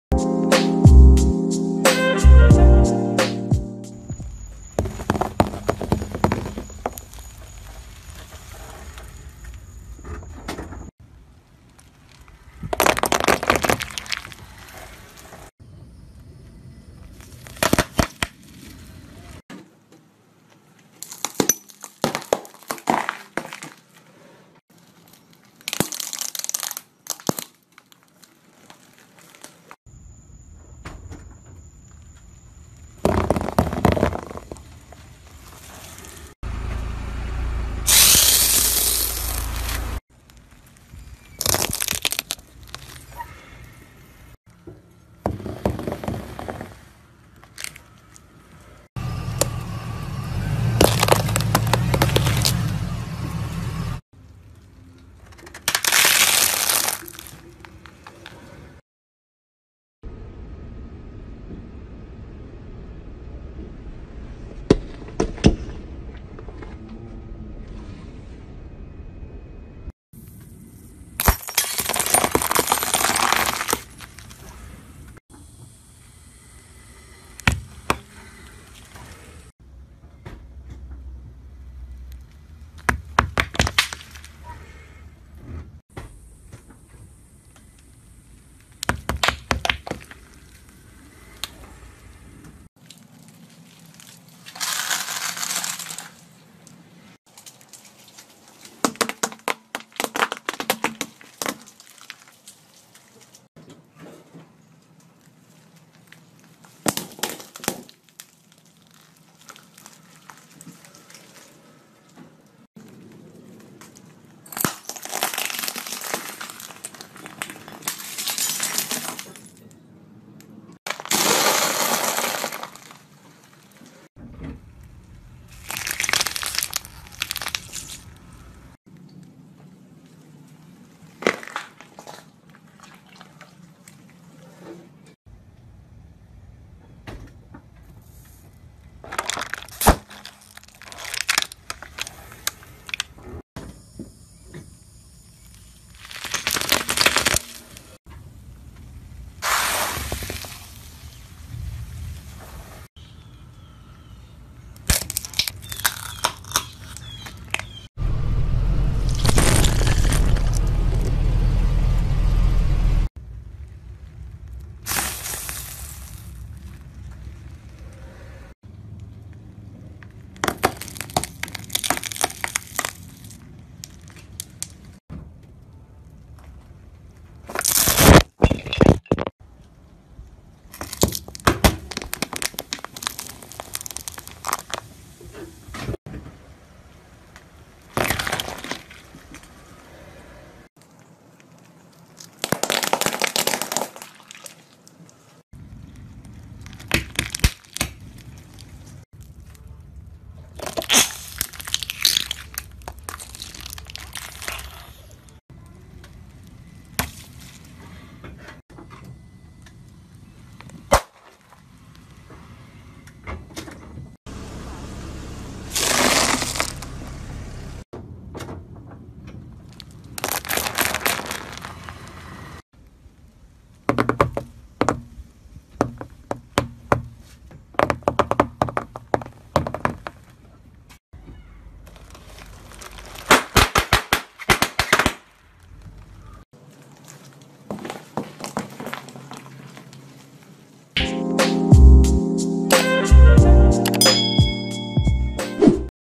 CAR CRUSHING STUFF ASMR
Crushing Crunchy & Soft Things by Car! - Bath boom, Floral Foam, Paint Balls and More!

Some of the most interesting videos compilation. Like a boss. Best Cube, Top 10. And many more.